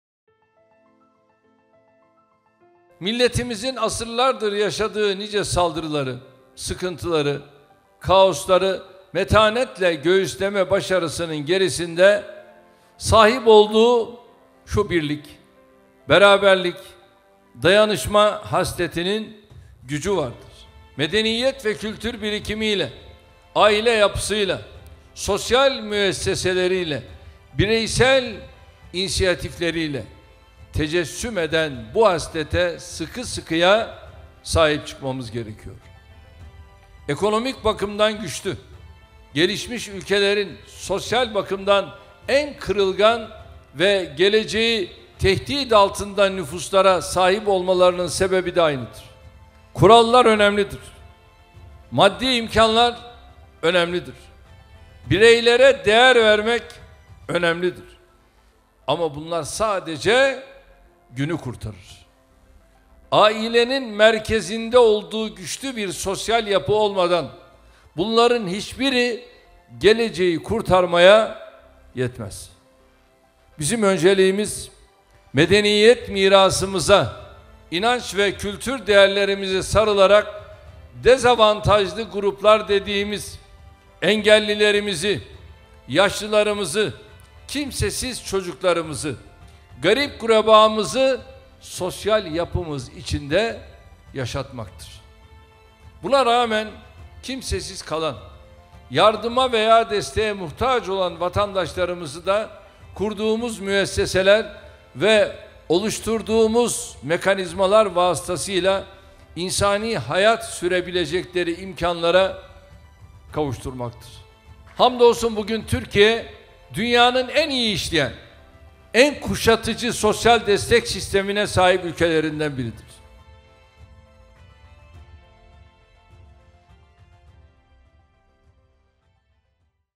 Cumhurbaşkanı Erdoğan, Darülaceze Sosyal Hizmet Şehri temel atma törenine katıldı
Cumhurbaşkanı Erdoğan: “Bugün Türkiye, dünyanın en iyi işleyen, en kuşatıcı sosyal destek sistemine sahip ülkelerinden biridir.”